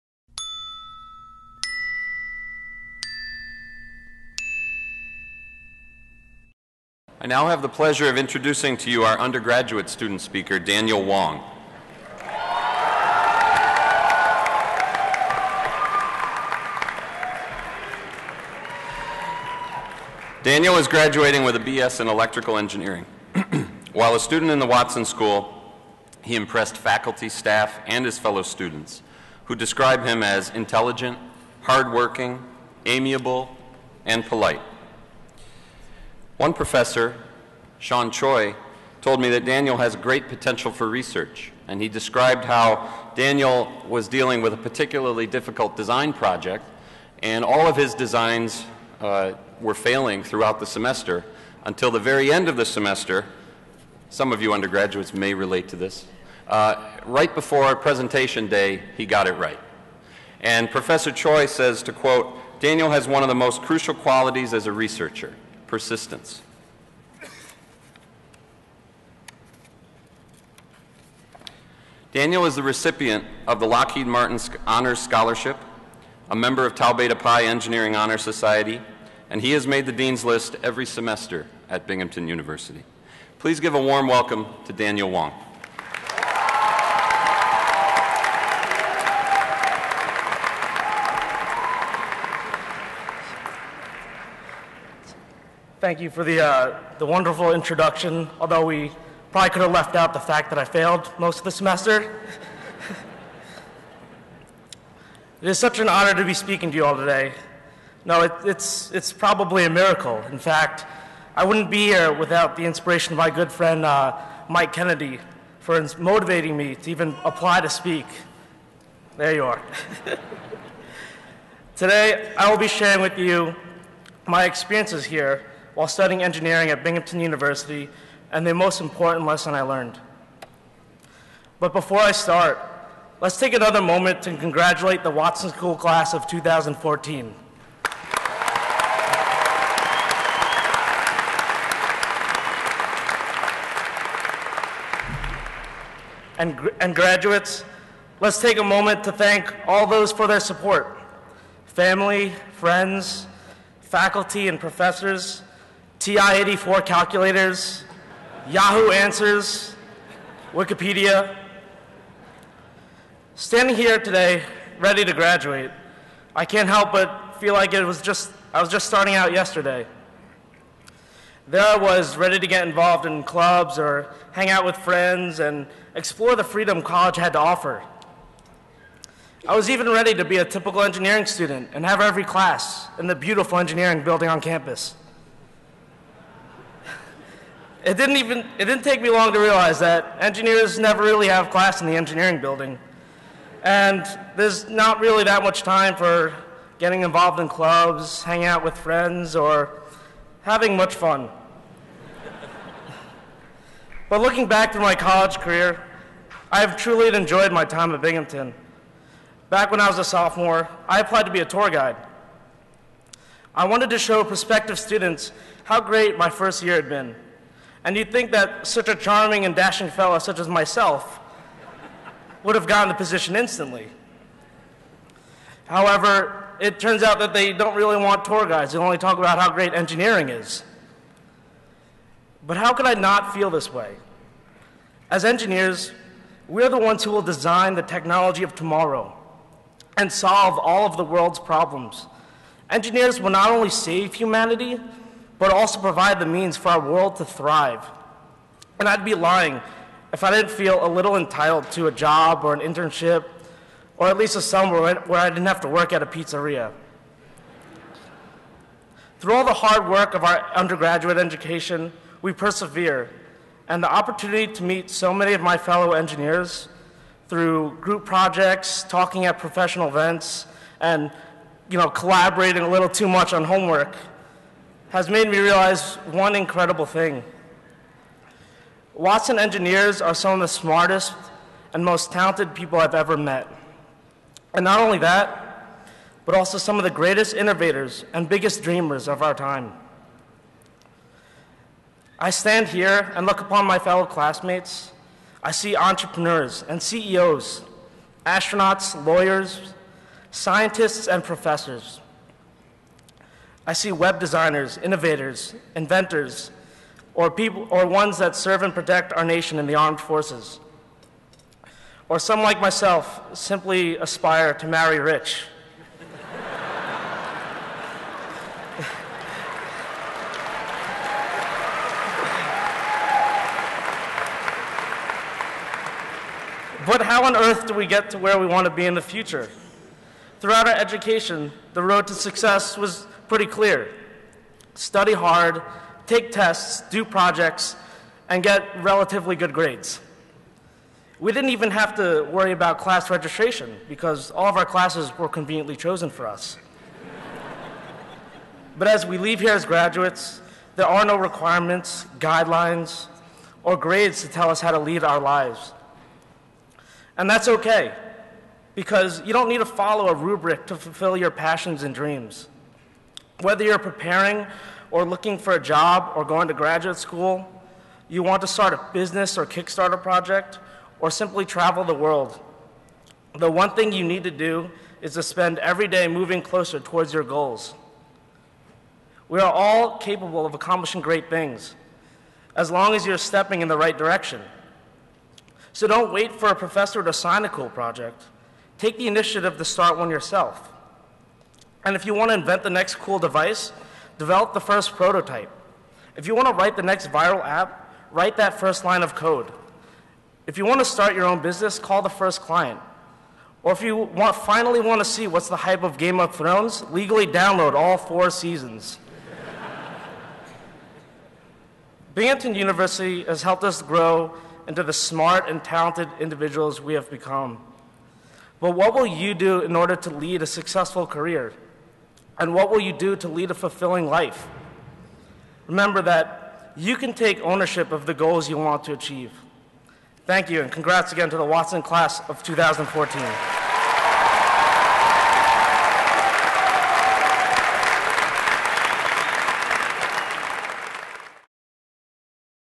Binghamton University Commencement Spring 2014 - Daniel Wong "Take Ownership of Your Goals"
Speech from the Spring 2014 commencement proceedings at Binghamton University

Thomas J. Watson School of Engineering and Applied Science -- 8:30 a.m. Saturday, May 17, Events Center.

Daniel Wong, from Merrick, N.Y., is receiving his bachelor's degree in electrical engineering. He has been on the Dean's List every semester, carries a cumulative grade point average of 3.71, and was admitted to Tau Beta Pi Engineering Honor Society as a junior. Recipient of the Lockheed Martin Honors Scholarship, he traveled to New Orleans over two spring breaks to construct homes as a member of Habitat for Humanity, and it an active member of the Alpha Kappa Psi Co-Ed Professional Business Fraternity. He has also been a student assistant in the Watson Career and Alumni Connections Office, working with students to develop skills to aid them in their job searches and careers. He begins a position as a fiber optics test manufacturing engineer with a firm in Allentown, Pa. in June.